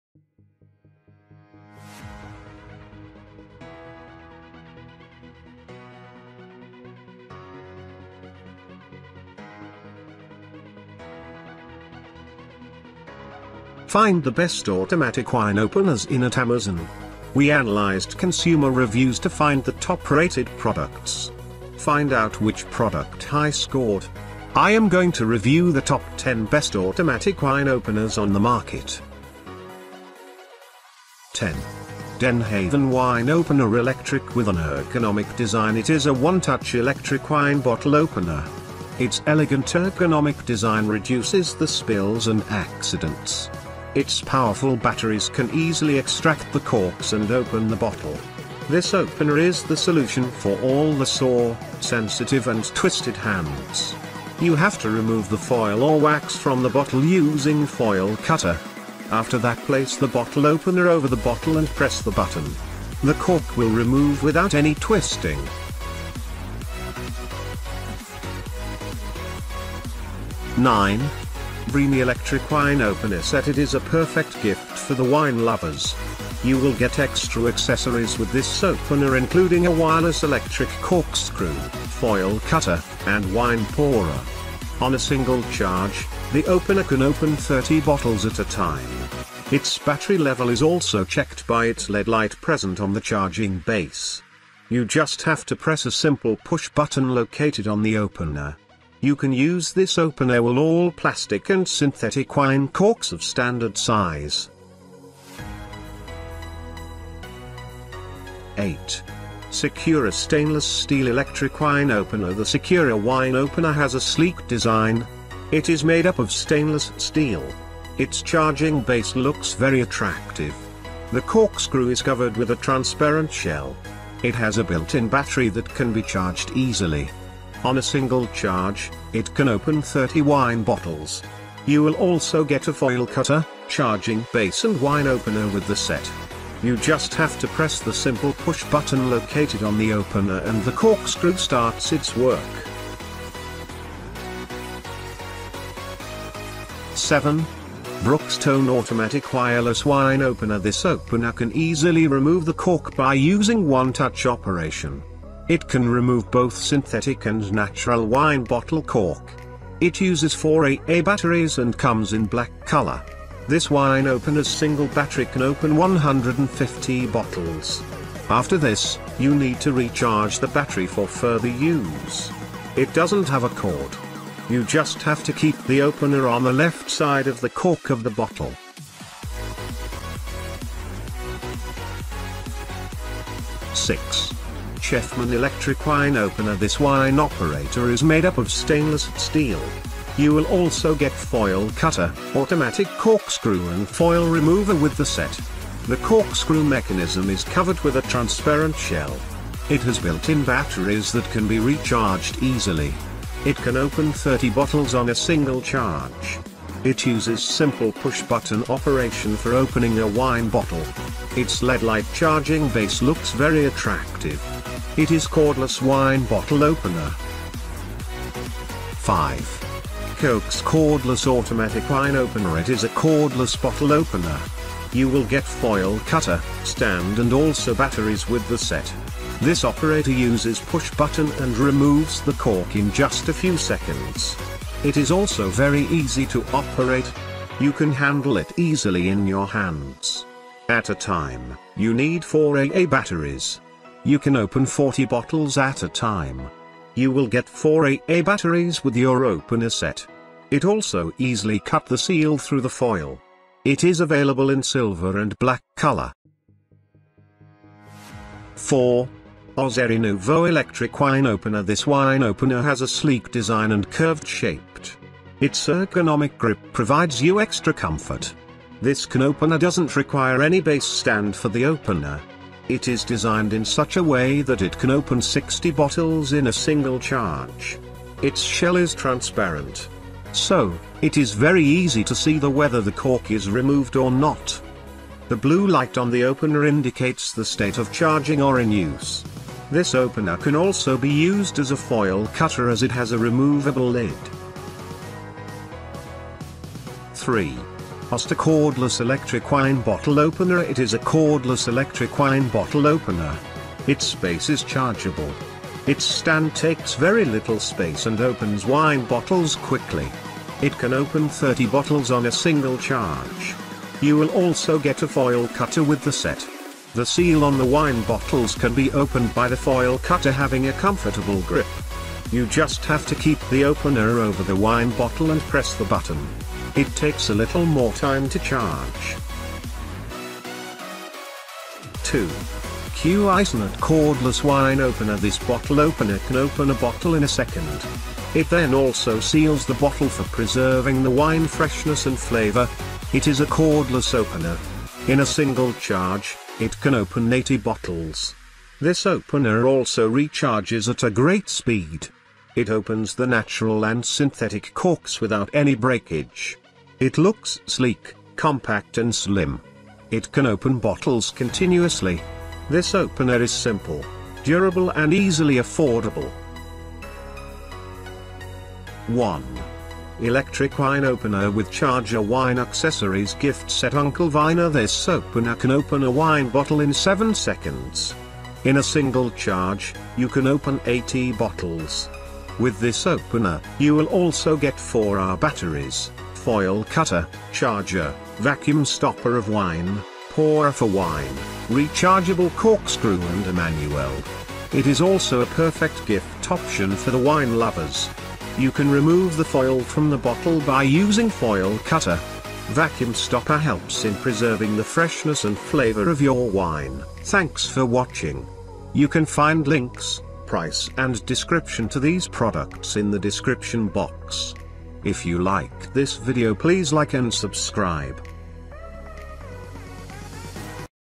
Top 10: Best Automatic Wine Opener in 2023
Hi guys, I am going to show video review on the top 10 Best Automatic Wine Opener on the market. Get your Best Automatic Wine Openers from here

Top 10 Best Automatic Wine Openers in 2019